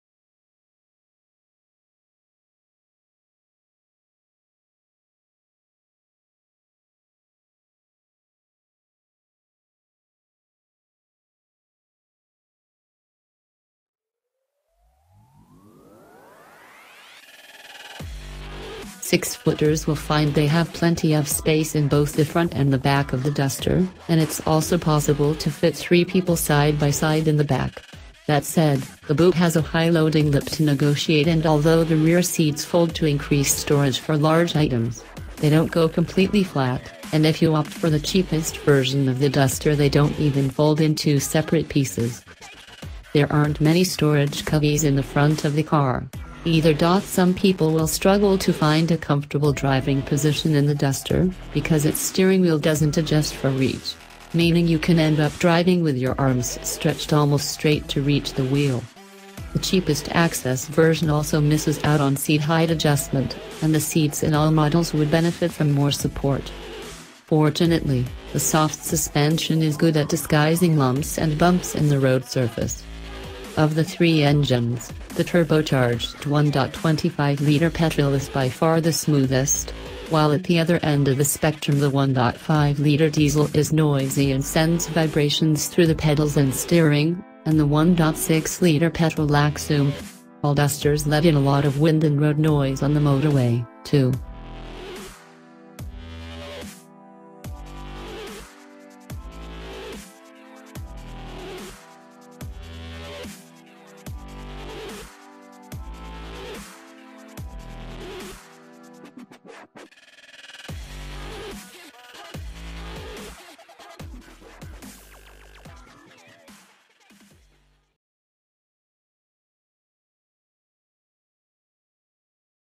Hot News !!! Dacia Duster 2018 Cheap but Really Safe Car
Hot News !!! Dacia Duster 2018 Cheap but Really Safe Car - Six-footers will find they have plenty of space in both the front and the back of the Duster, and it’s also possible to fit three people side-by-side in the back. That said, the boot has a high loading lip to negotiate and although the rear seats fold to increase storage for large items, they don’t go completely flat – and if you opt for the cheapest version of the Duster they don’t even fold in two separate pieces.

There aren’t many storage cubbies in the front of the car, either.Some people will struggle to find a comfortable driving position in the Duster, because its steering wheel doesn’t adjust for reach, meaning you can end up driving with your arms stretched almost straight to reach the wheel. The cheapest “Access” version also misses out on seat-height adjustment, and the seats in all models would benefit from more support.

Fortunately, the soft suspension is good at disguising lumps and bumps in the road surface.

Of the three engines, the turbocharged 1.25-litre petrol is by far the smoothest, while at the other end of the spectrum the 1.5-litre diesel is noisy and sends vibrations through the pedals and steering, and the 1.6-litre petrol lacks oomph. All Dusters let in a lot of wind and road noise on the motorway, too.

██████ GET DAILY UPDATES! NOW! ██████